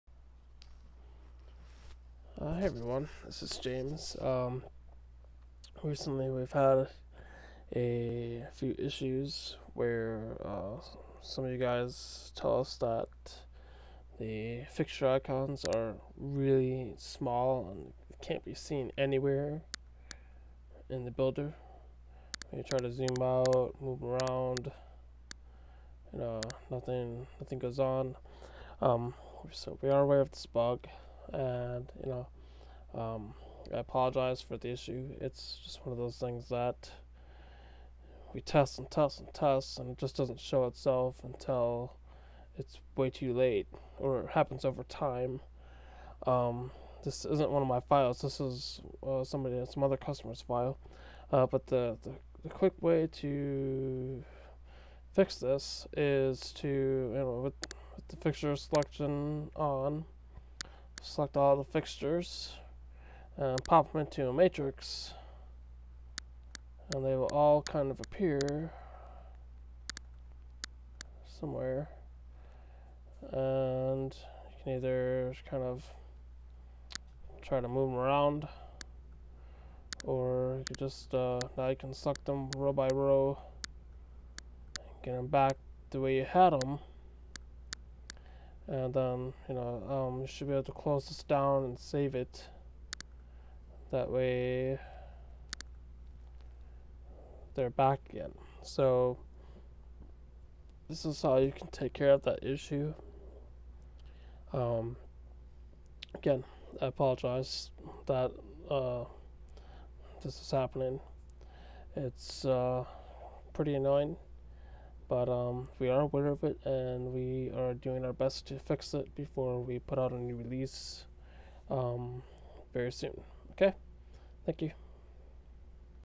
Fix the disappearing fixtures bug in MyDMX 2.0
In this quick video I show you how to get your fixtures back into the viewable area in the scene builder. sorry for the bug! We will fix it ASAP!